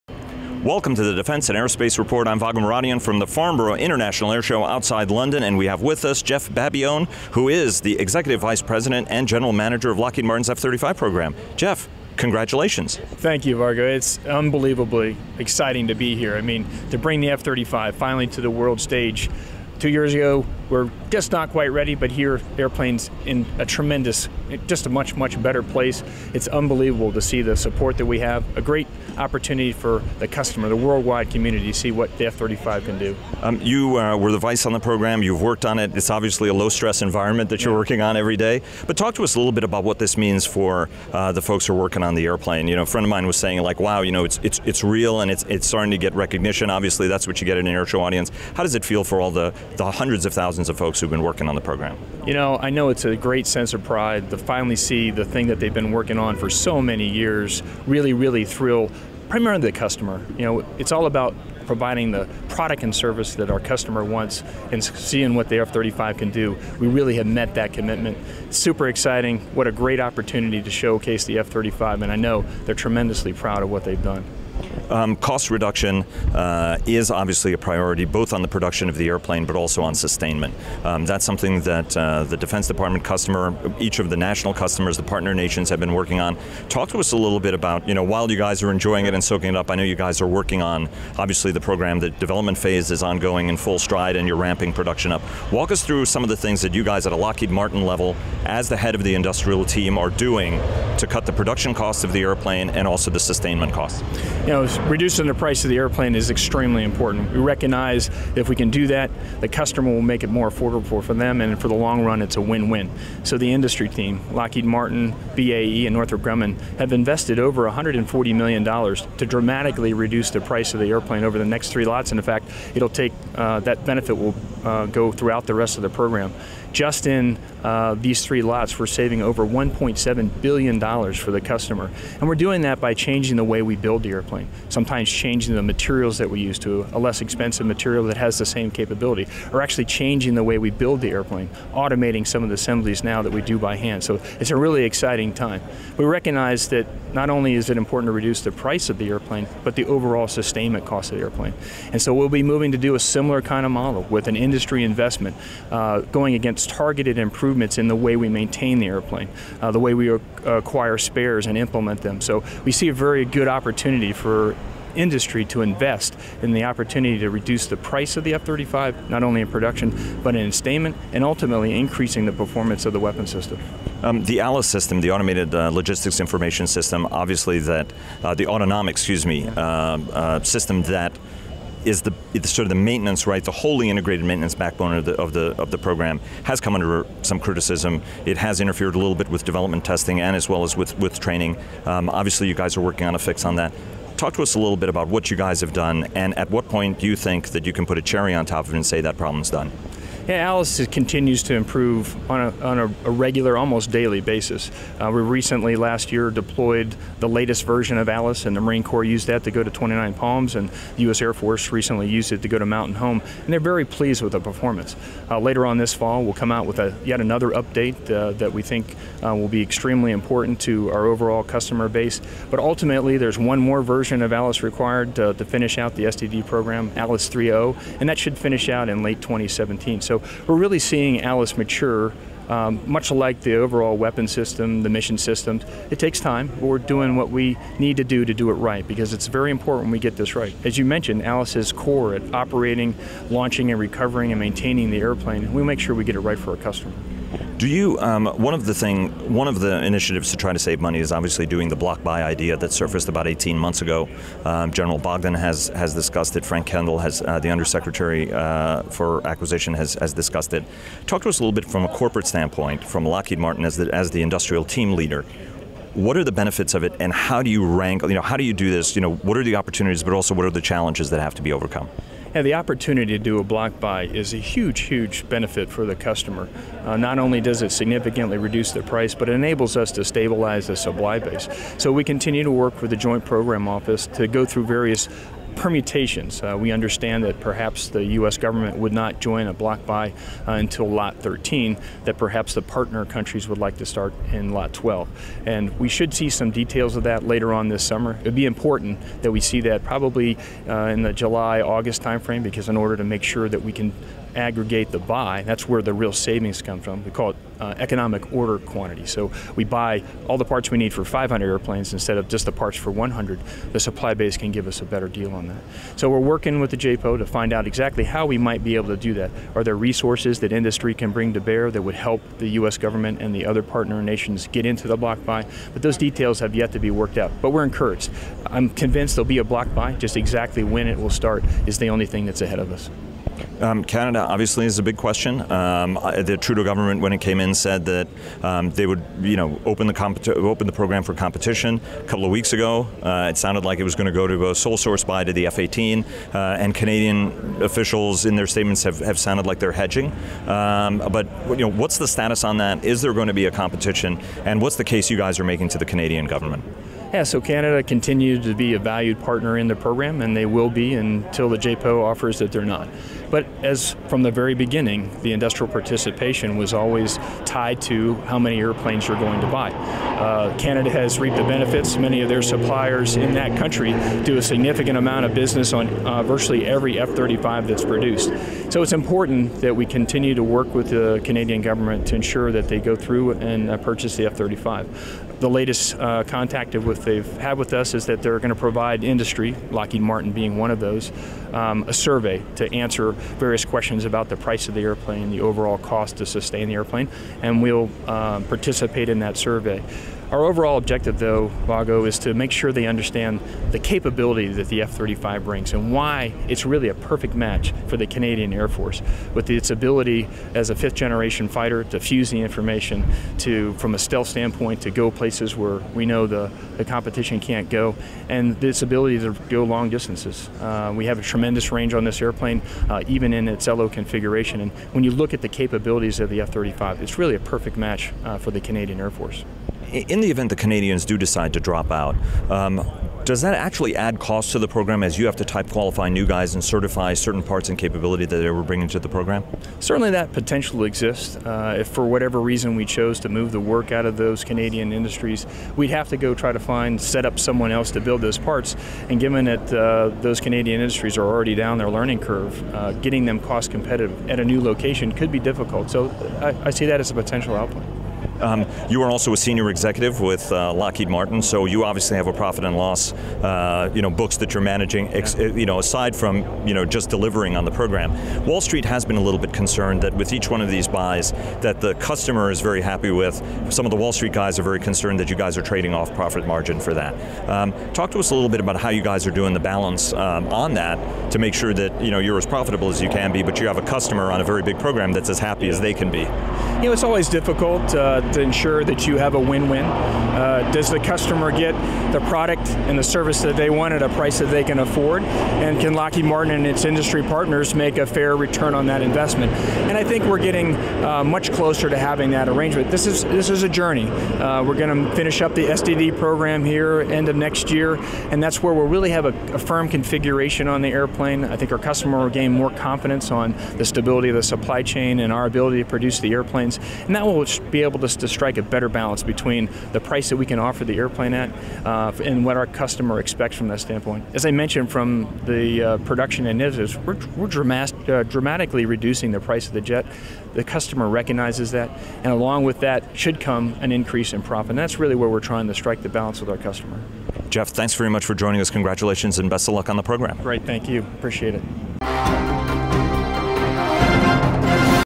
Lockheed Martin Works to Cut F-35 Purchase & Sustainment Costs
Jeff Babione, the Executive VP & General Manager for the F-35 program at Lockheed Martin, discusses what’s next for the Lightning II, cutting manufacturing and operating costs, Canada’s fighter competition, and more.